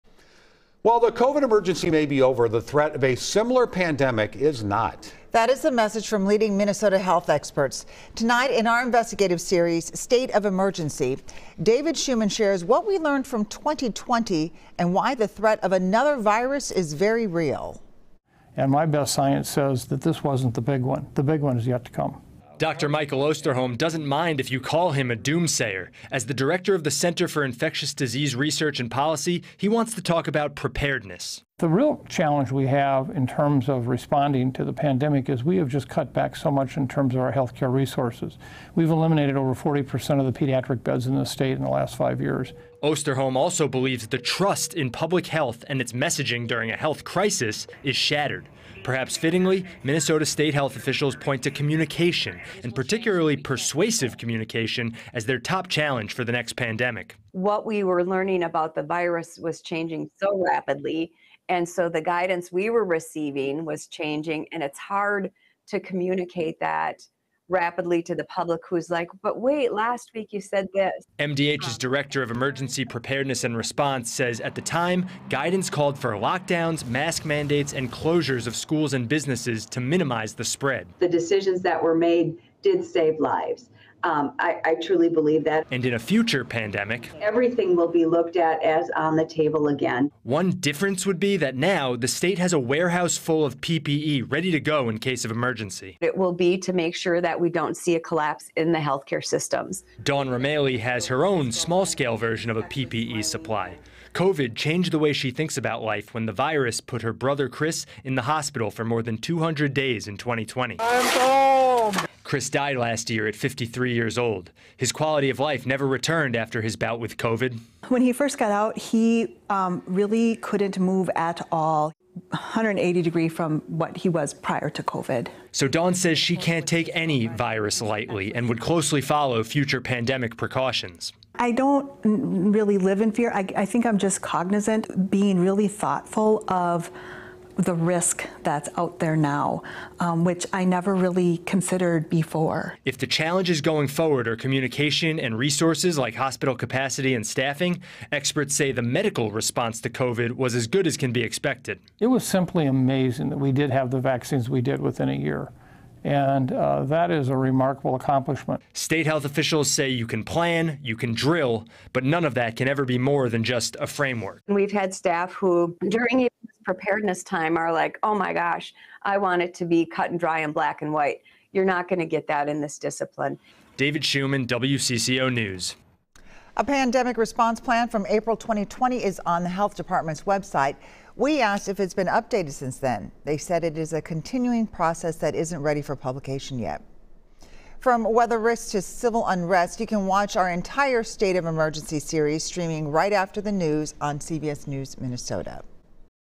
WCCO Investigates: State of Emergency: Is Minnesota prepared for the NEXT pandemic?
WCCO's David Schuman speaks with experts in Minnesota's medical community to answer important questions regarding the possibility of another pandemic.